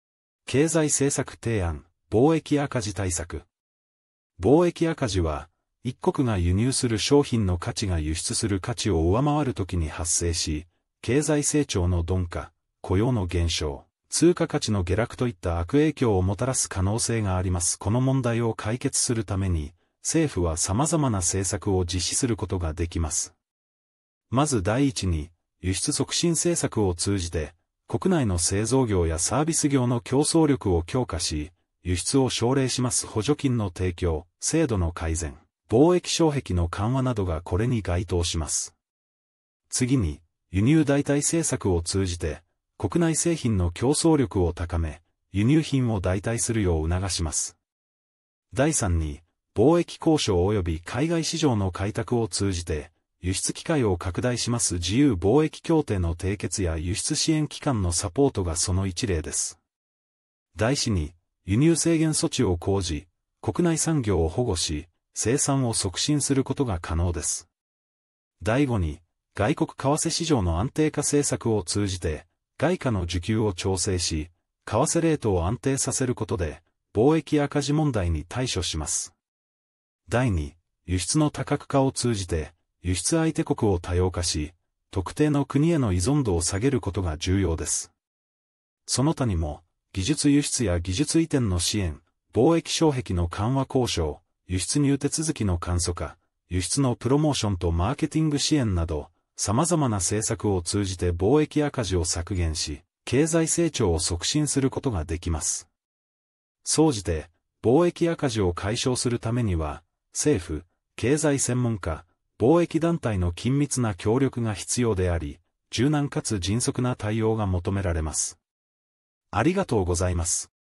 [経済政策提案] 貿易赤字対策
貿易赤字は、一国が輸入する商品の価値が輸出する価値を上回るときに発生し、経済成長の鈍化、雇用の減少、通貨価値の下落といった悪影響をもたらす可能性があります。この問題を解決するために、政府はさまざまな政策を実施することができます。

まず第一に、輸出促進政策を通じて、国内の製造業やサービス業の競争力を強化し、輸出を奨励します。補助金の提供、制度の改善、貿易障壁の緩和などがこれに該当します。

次に、輸入代替政策を通じて、国内製品の競争力を高め、輸入品を代替するよう促します。

第三に、貿易交渉および海外市場の開拓を通じて、輸出機会を拡大します。自由貿易協定の締結や輸出支援機関のサポートがその一例です。

第四に、輸入制限措置を講じ、国内産業を保護し、生産を促進することが可能です。

第五に、外国為替市場の安定化政策を通じて、外貨の需給を調整し、為替レートを安定させることで、貿易赤字問題に対処します。

第六に、輸出の多角化を通じて、輸出相手国を多様化し、特定の国への依存度を下げることが重要です。

その他にも、技術輸出や技術移転の支援、貿易障壁の緩和交渉、輸出入手続きの簡素化、輸出のプロモーションとマーケティング支援など、さまざまな政策を通じて貿易赤字を削減し、経済成長を促進することができます。

総じて、貿易赤字を解消するためには、政府、経済専門家、貿易団体の緊密な協力が必要であり、柔軟かつ迅速な対応が求められます。

日本,経済政策提案,経済,政策,提案,貿易赤字対策,貿易,赤字,対策

この映像は次のサイトを利用して作りました。